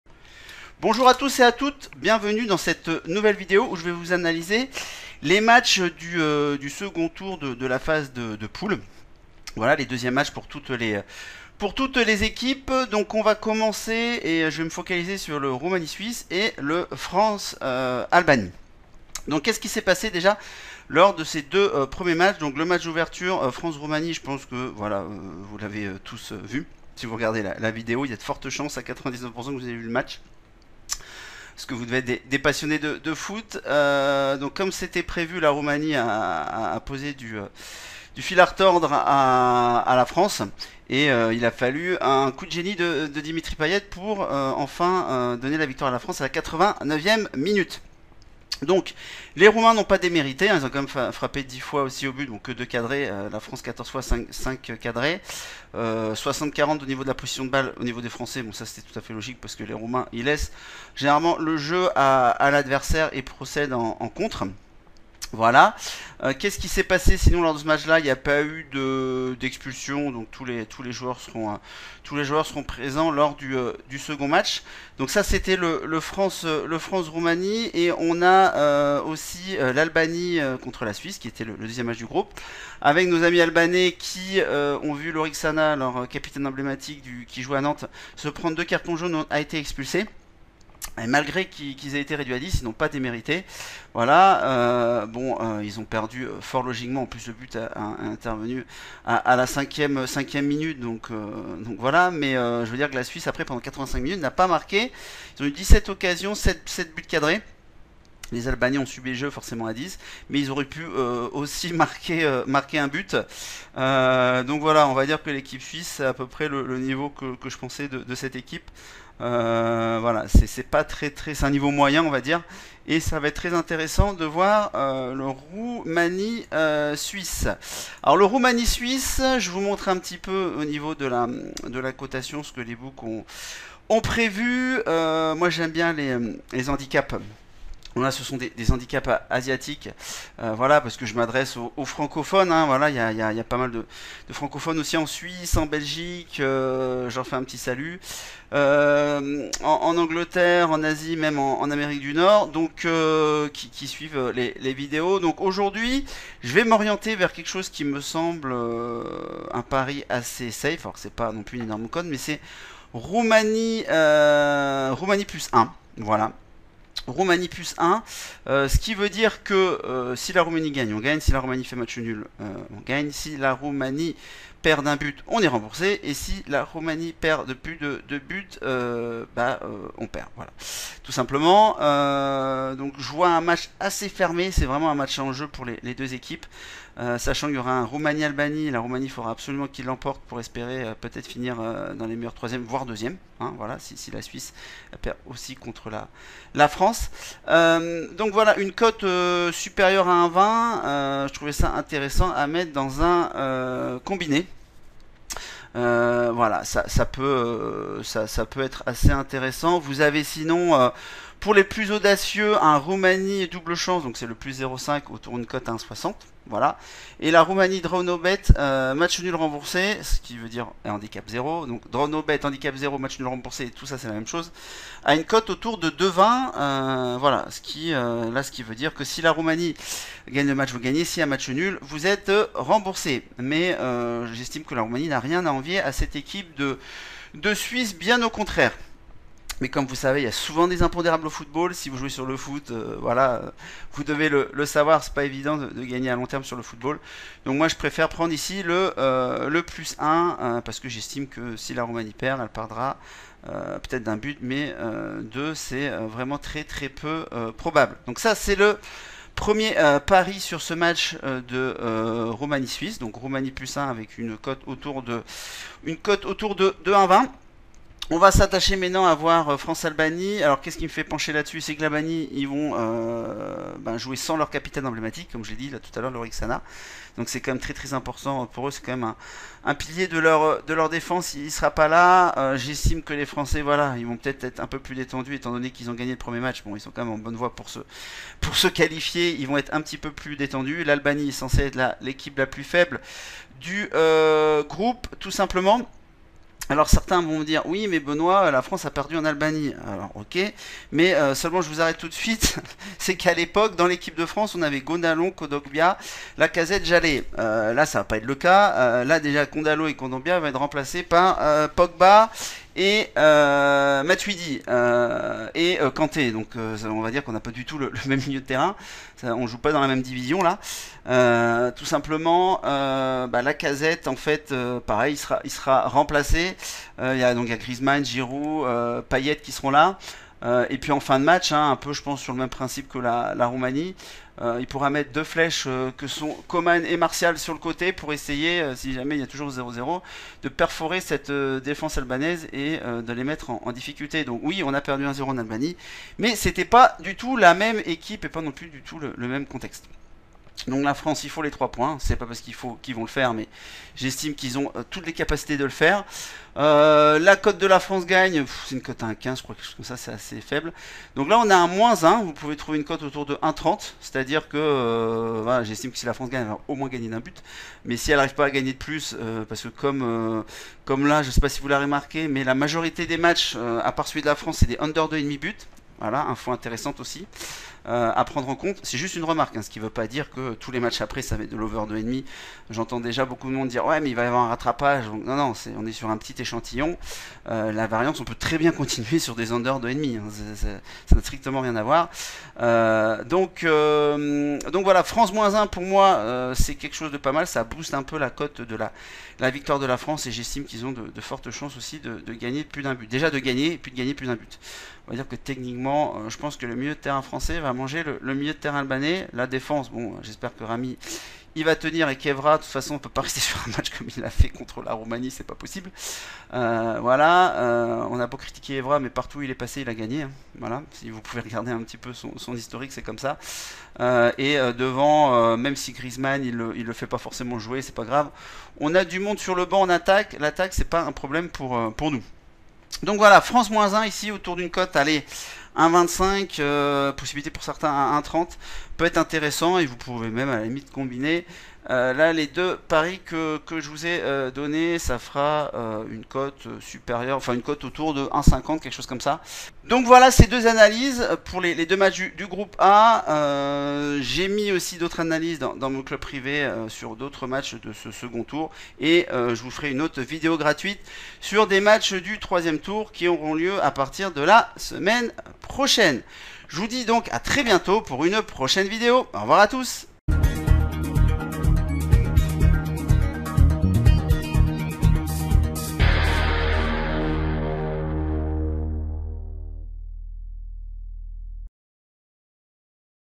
EURO 2016 : France-Albanie et Roumanie-Suisse . Sur qui parier ?
France-Albanie et Roumanie-Suisse : sur qui parier ?

Je vous dévoile mes analyses sur les matchs du groupe A : France-Albanie et Roumanie-Suisse et vous donne deux paris intéressants à prendre !
Benoit Treanton. Parieur pro, formateur en paris sportifs et auteur du livre Comment Vaincre la Chance